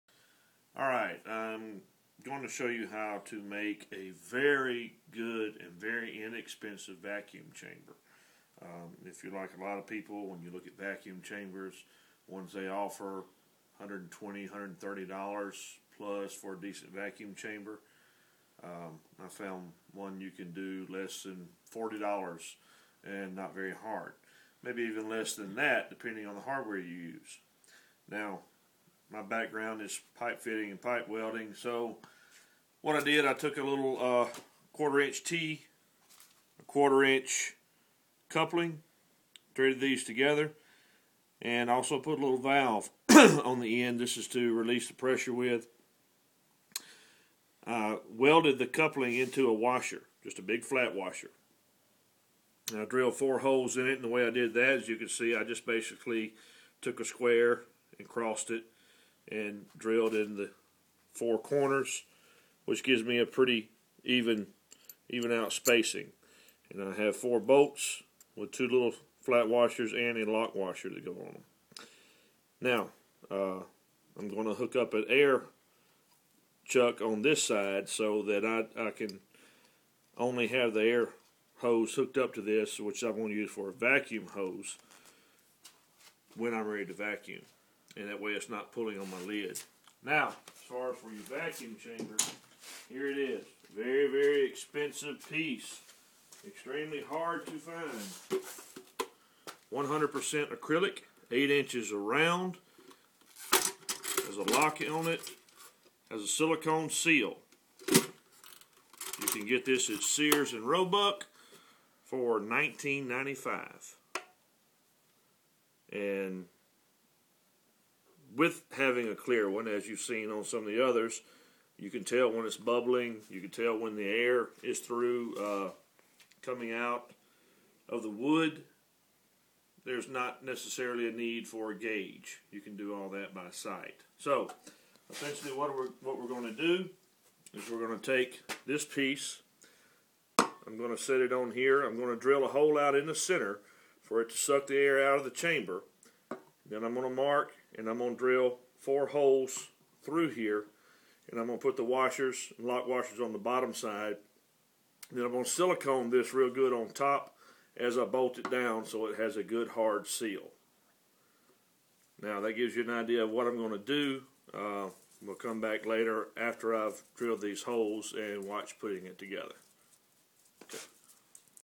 Making a stabilizing vacuum chamber
Easy an inexpensive way to make a good vacuum chamber